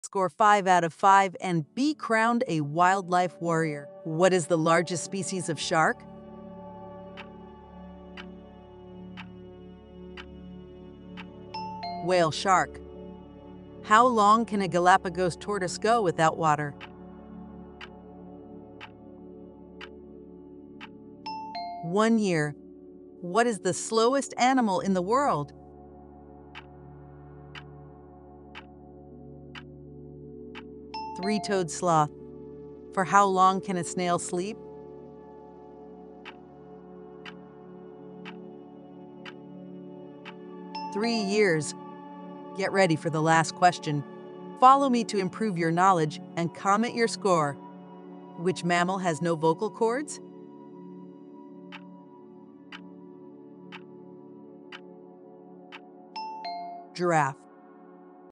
Animal Trivia: Can You Get 5/5 Correct in this Animal Trivia Quiz?🦁🧠| Brain Tinkering
Did you get 5/5 Correct? Share with a friend! And let me know how you did in the comments!

Embark on a safari of knowledge with Brain Tinkering! 🌍🐾 In our Animal Trivia Challenge, we explore the fascinating world of animals through mind-bending trivia. From the fastest land animals to the depths of the ocean, test your animal IQ and discover the incredible diversity of the animal kingdom. Ready to roar with knowledge? Hit play now!